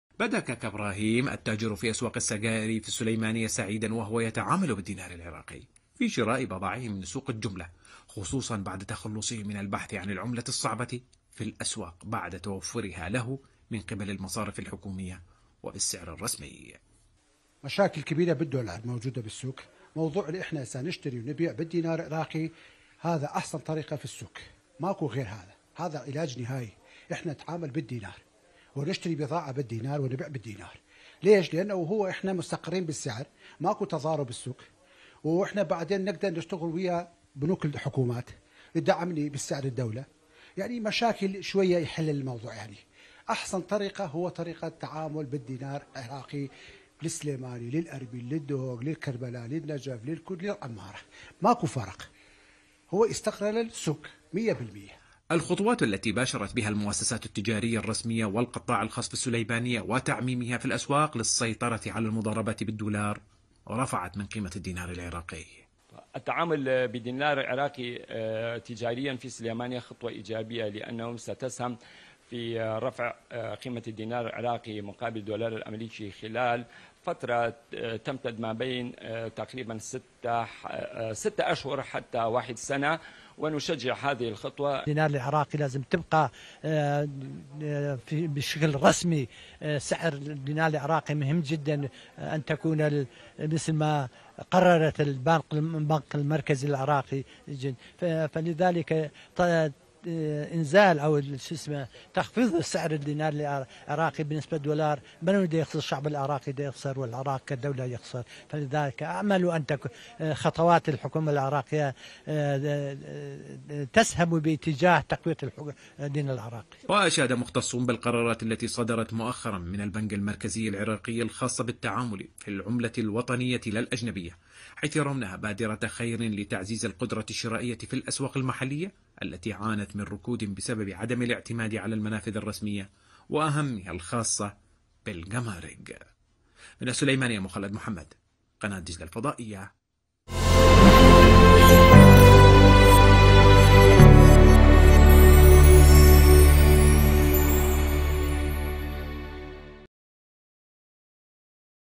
تجار في سليمانية: التعامل بالدينار العراقي رفع من قيمته في الأسواق المحلية
11258📺 افقي